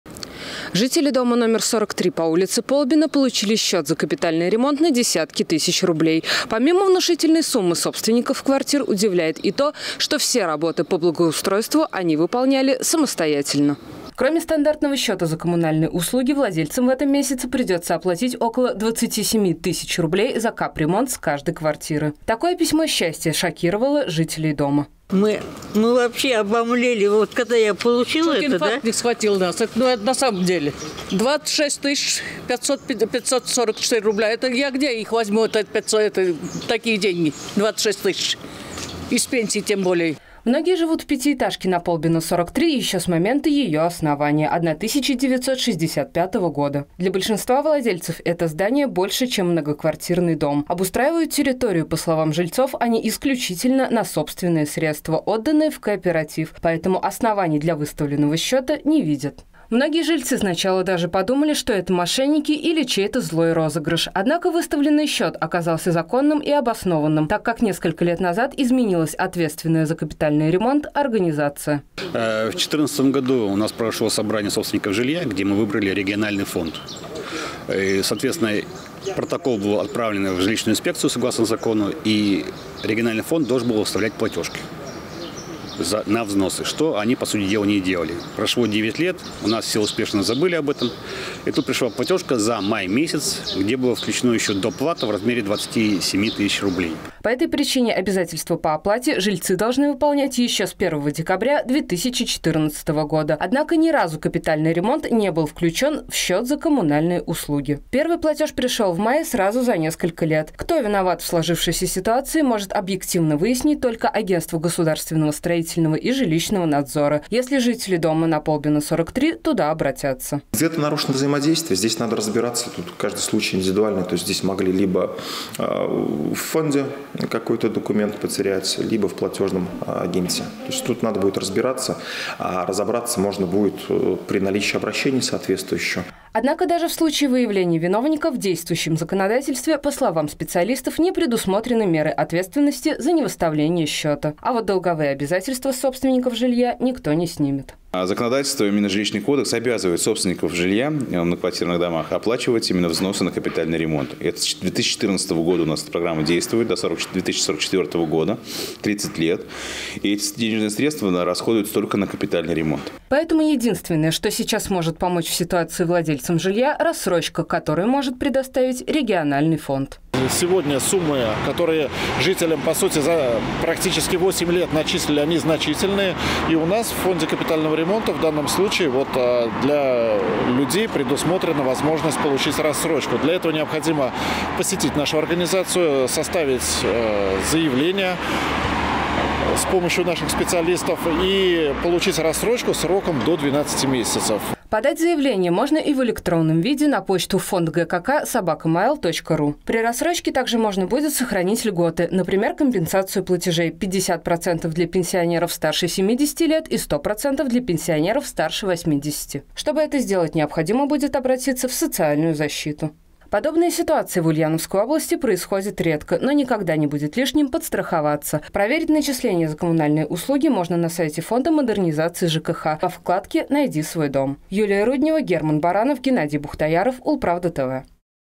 По 27 000 рублей с квартиры. За что жителям дома на Полбина выставили огромные счета
Может ли счёт за коммунальные услуги размером с зарплату и больше, чем пенсия? К сожалению, в Ульяновской области - может.  В этом месяце неприятный сюрприз ждал жителей Засвияжского района. Подробности - далее.